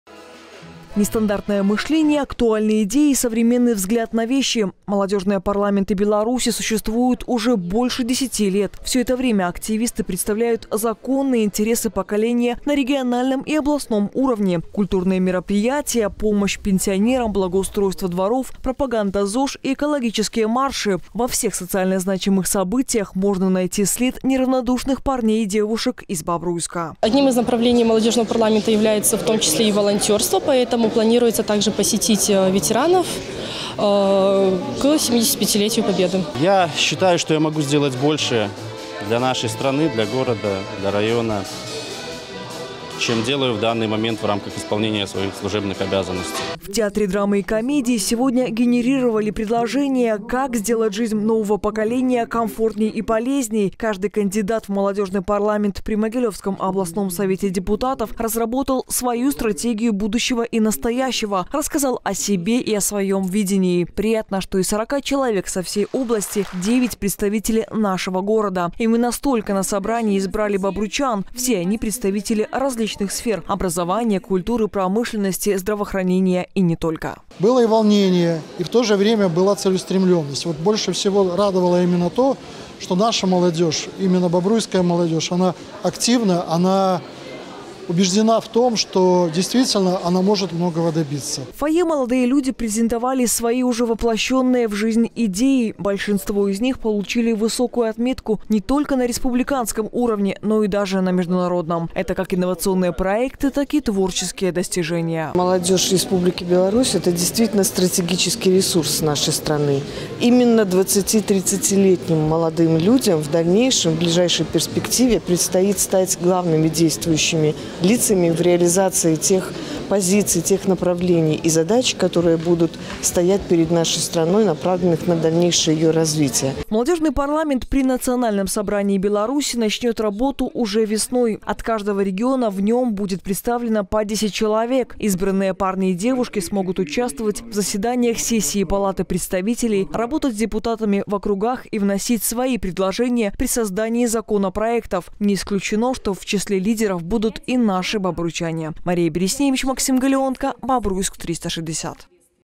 Кто в числе лидеров?
Уже весной в стране появится Молодежный парламент при Национальном Собрании. Основной мотив правительства – задействовать юных и талантливых в проектах государственной значимости.